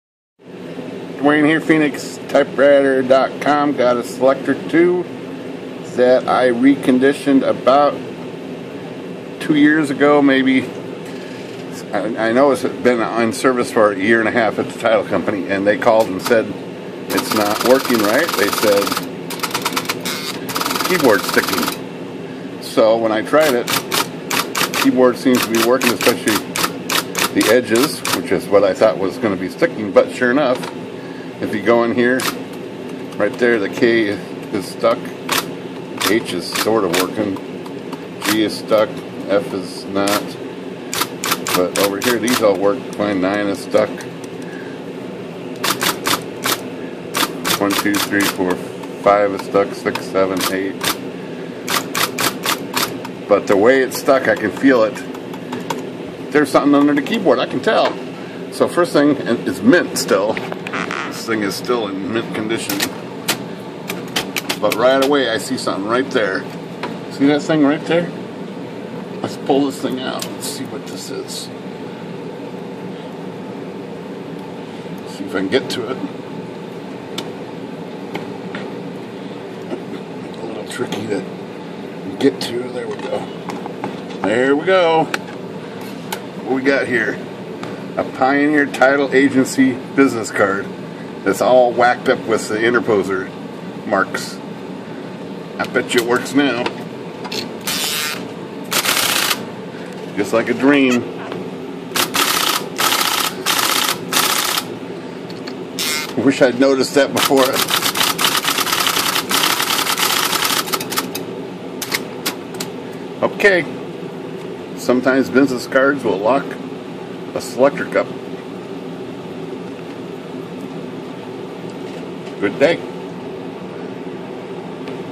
IBM Selectric Typewriter Keyboard Locked Up, Partially Frozen, Analyzed and Repaired
Found an easy solution to this repair